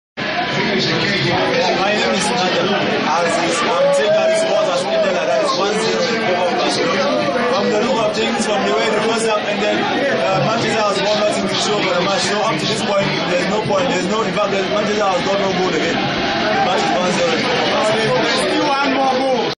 La Finale de la Champions League 2009 vue du Mboa
Réaction des supporters du Barca à la fin du match (4).avi